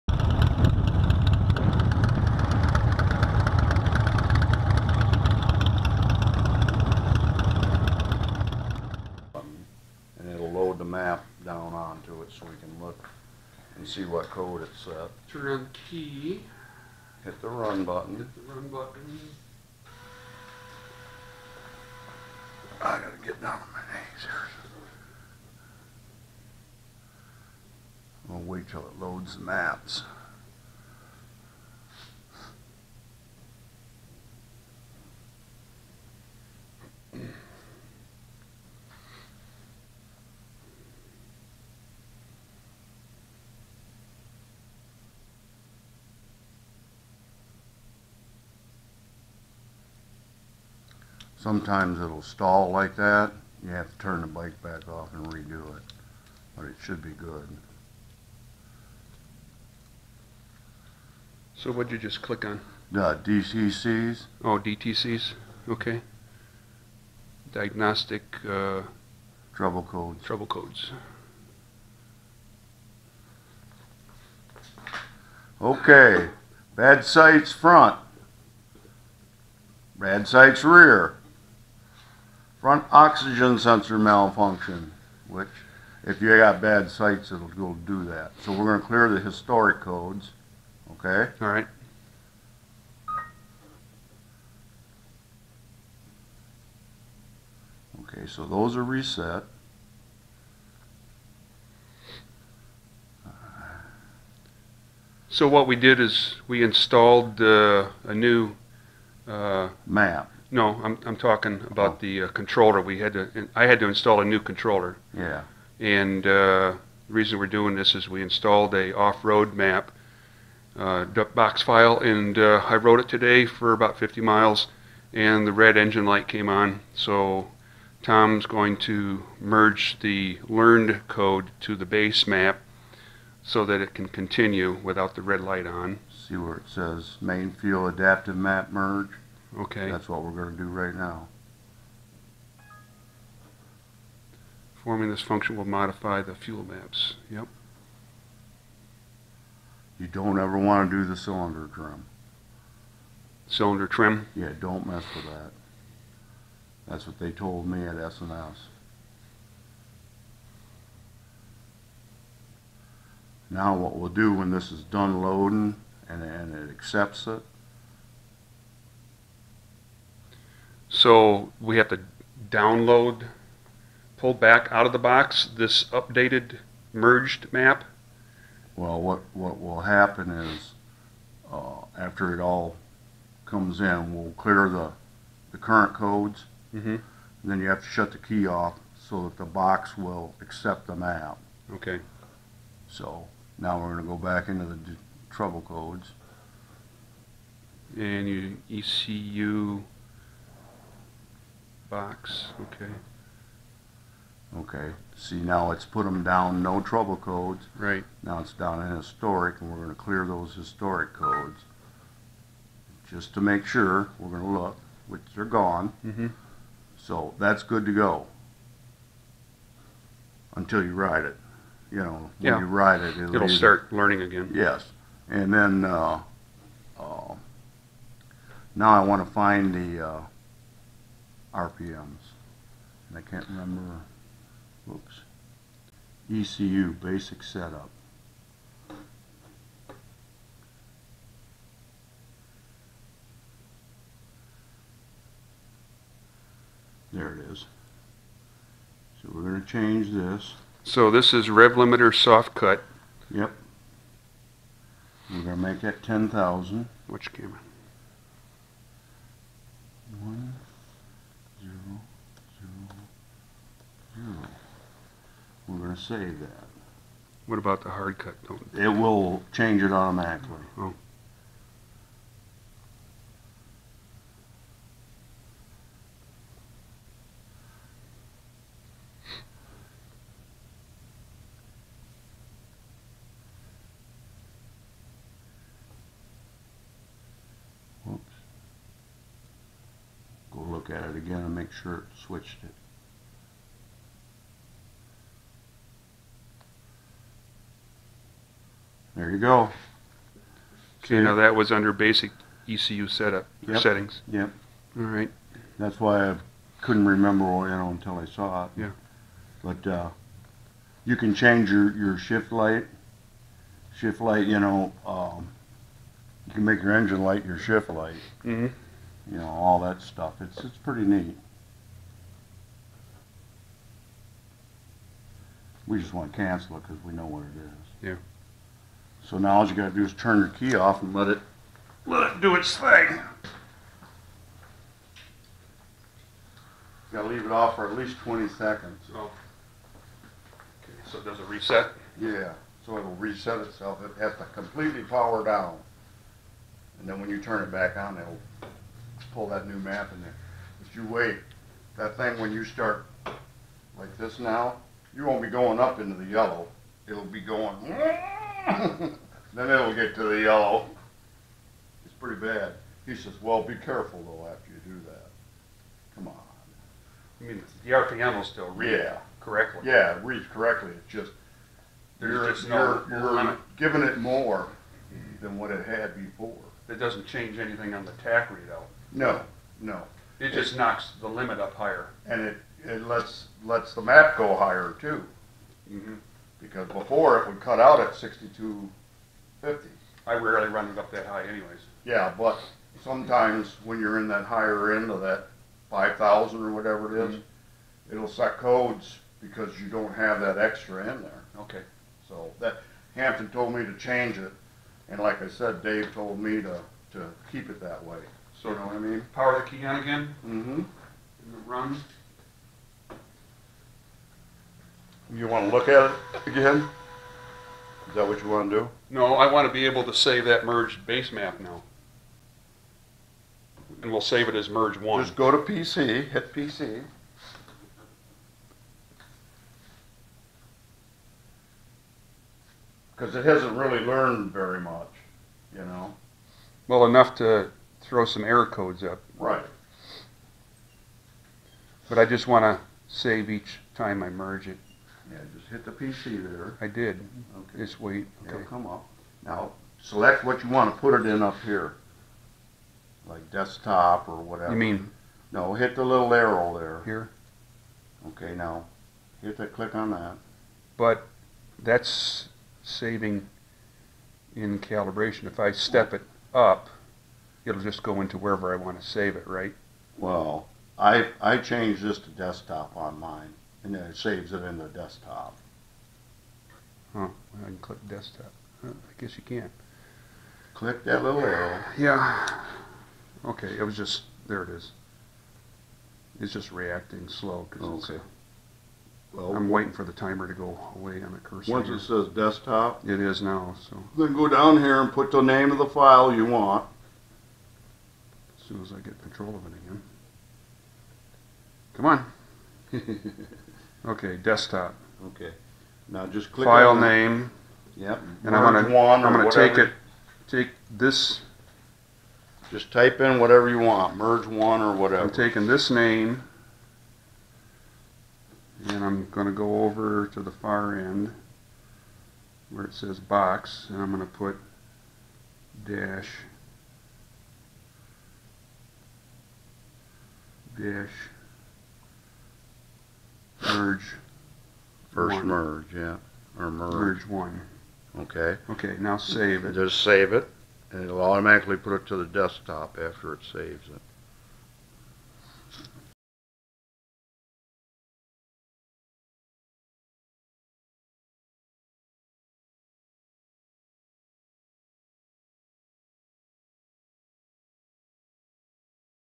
Bigdog ECM Map Merge
A "How To" video on merging your learned ECM data to the BASE MAP data for EFI Bigdog Motorcycles with Delphi/S&S ECM.